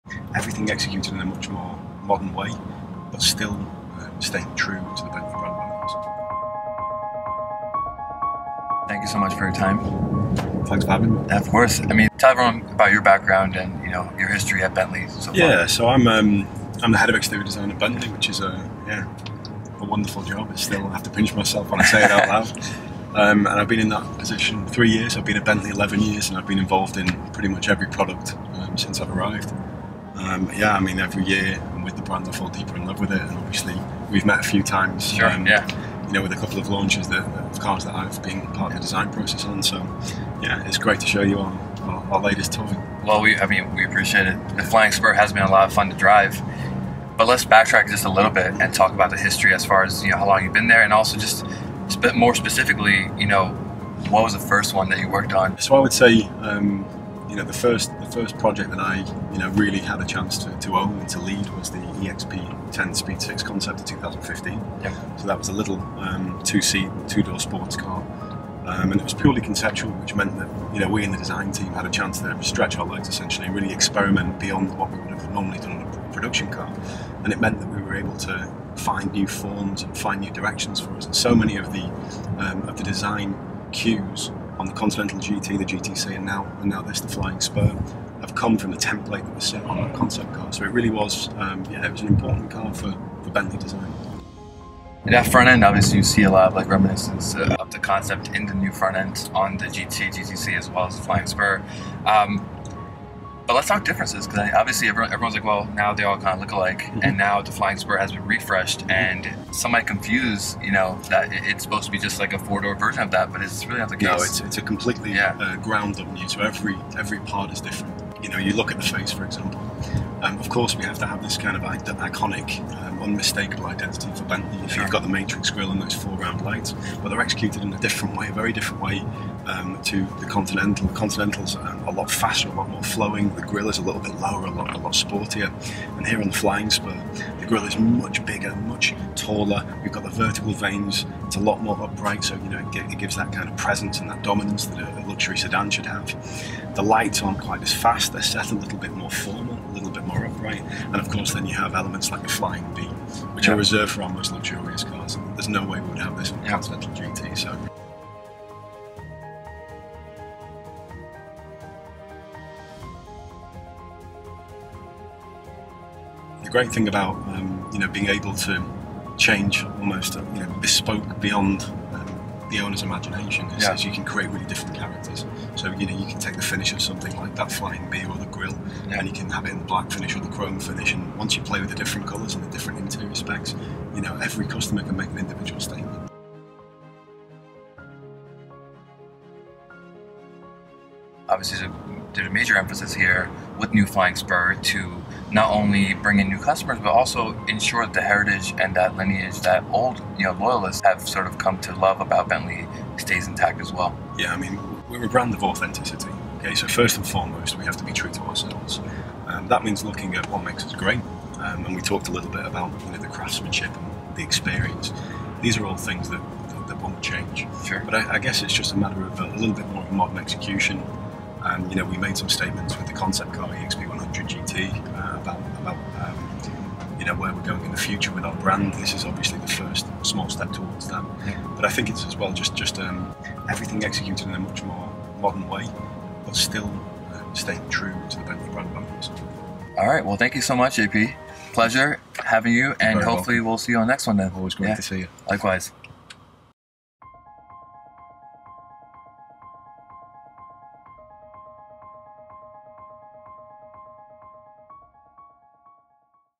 Driving the 2020 Flying Spur with Bentley's Head of Exterior Design
Atif Kazmi drives through the South of France with JP Gregory, Bentley’s Head of Exterior Design, as they talk about the all-new Flying Spur.

Introducing Years, a collection of stories rooted in cars and the lifestyle around them.